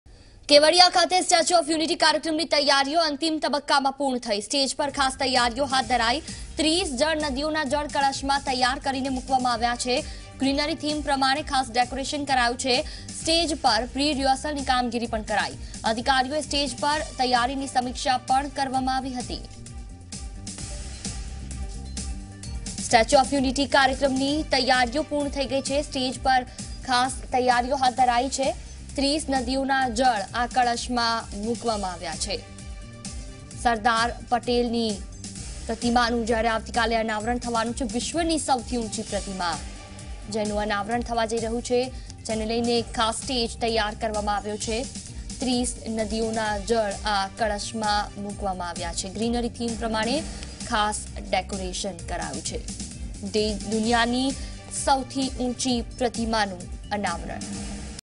Narmada: Preparations of Statue of Unity in final stage - Zee 24 Kalak
Narmada: Preparations of Statue of Unity in final stage